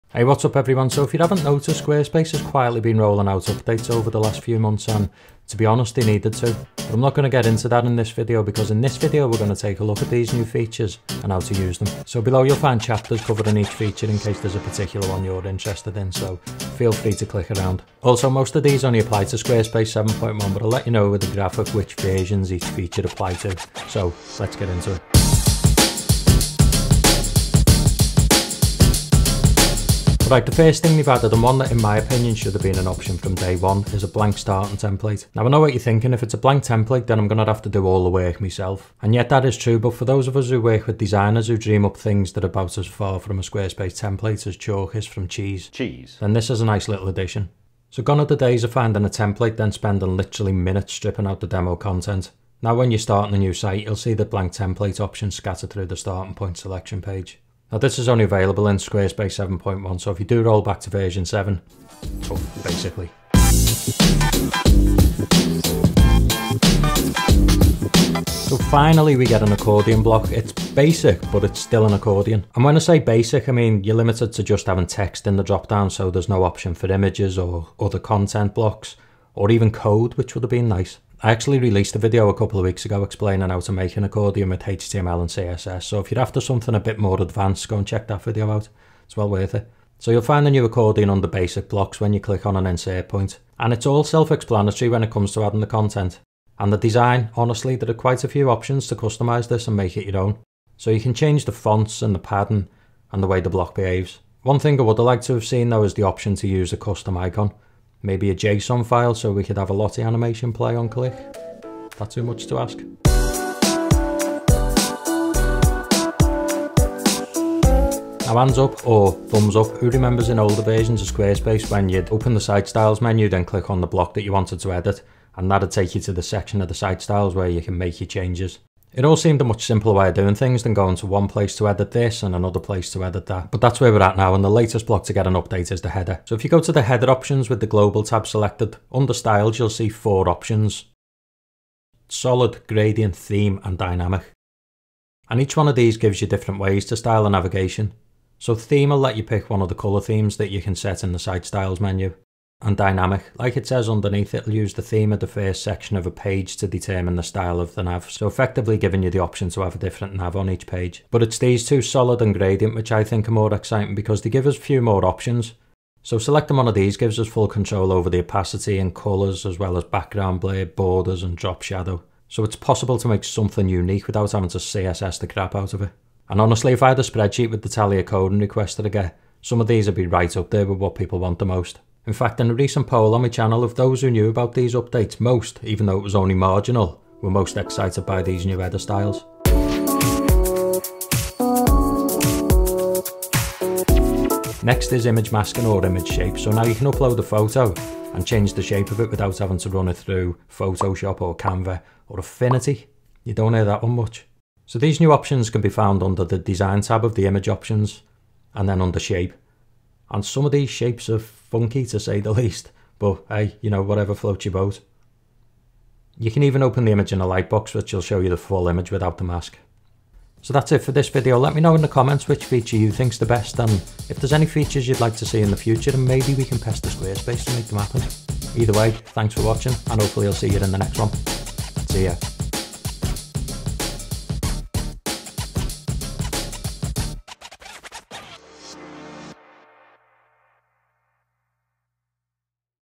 NEW Squarespace Updates EXPLAINED!
In this video we'll take a look at the new Squarespace updates for 2021 and the features it adds to your website.

Chapters
0:00 Intro
0:25 Blank Template
1:04 Accordion Block
1:53 Header Styles
3:09 Image Masks
3:40 Outro

If you've got any questions or requests for future videos drop them in the comments below 👇